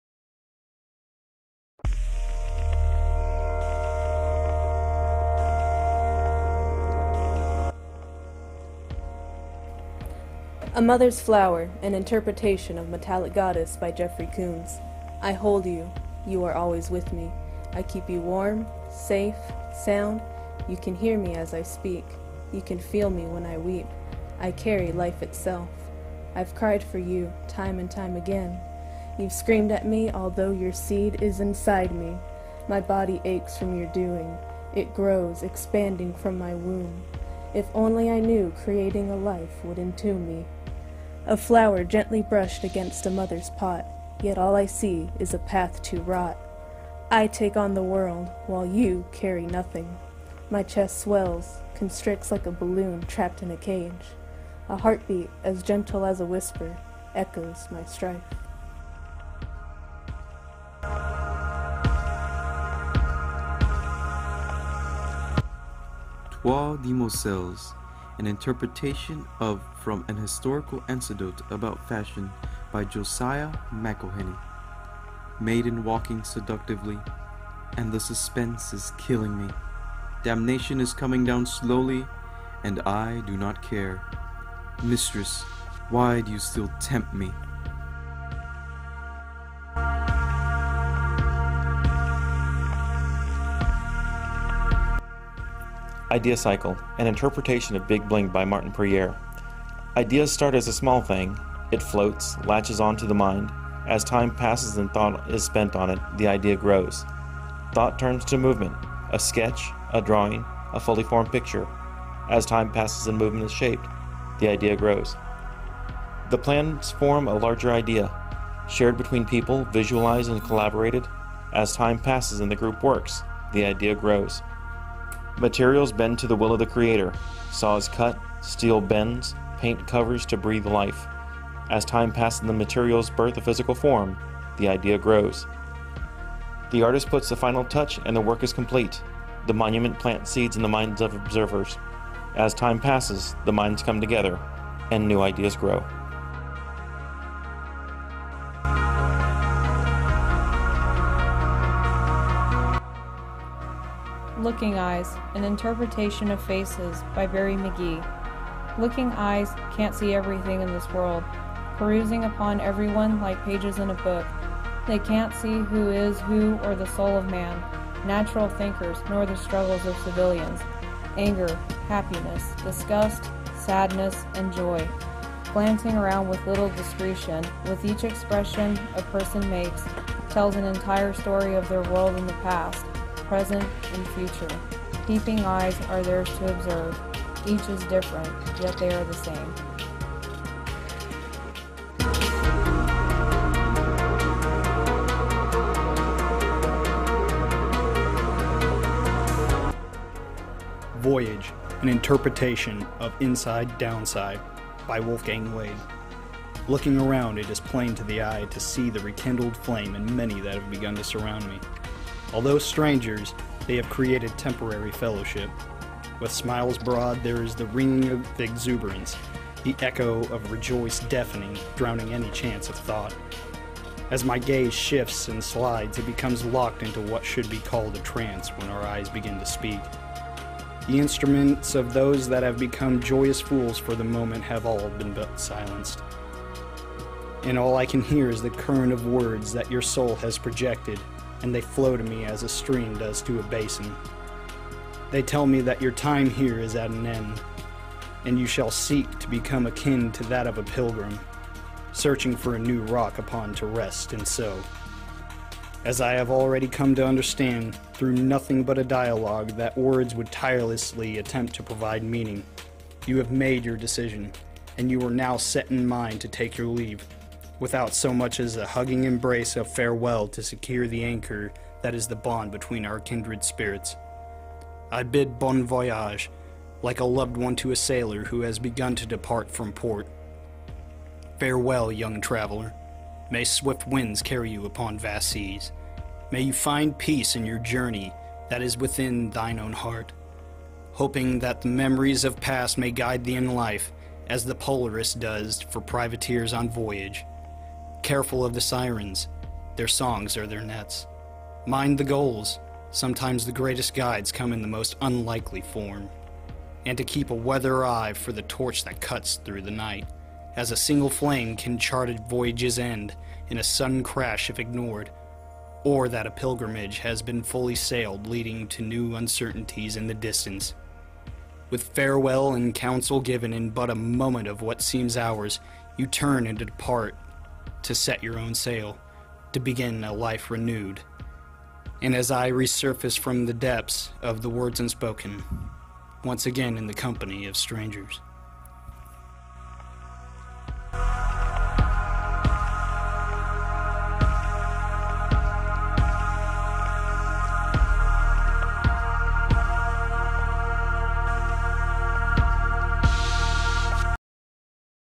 Team Primus Burton English 1302 Fall 2016
Dr. Burton's Ekphrasis project. English 1302
Alyssa Guidry, Courtney Hallman, Frank Murguia, Louis Meyer, Ryan McMahon